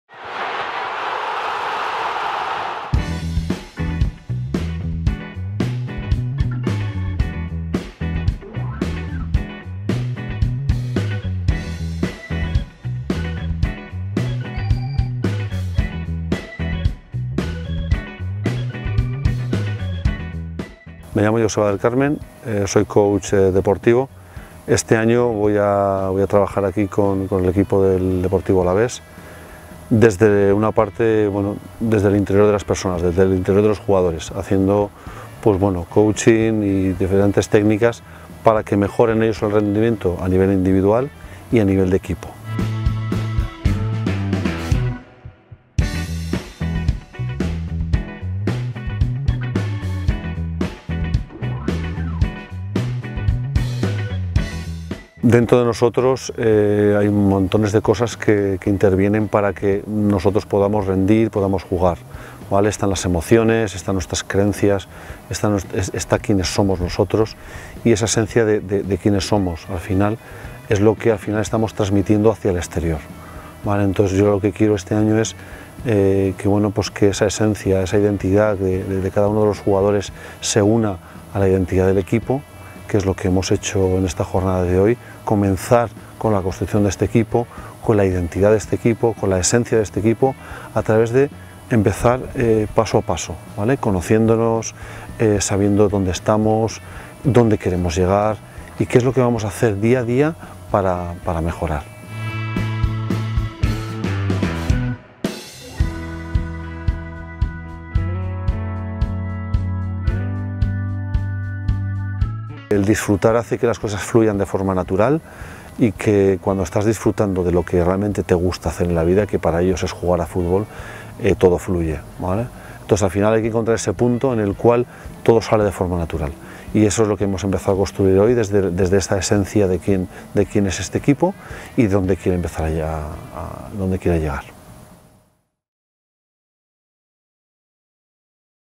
Haciendo equipo en Argómaniz
El Deportivo Alavés aprovecha la concentración de Argómaniz para realizar actividades de grupo.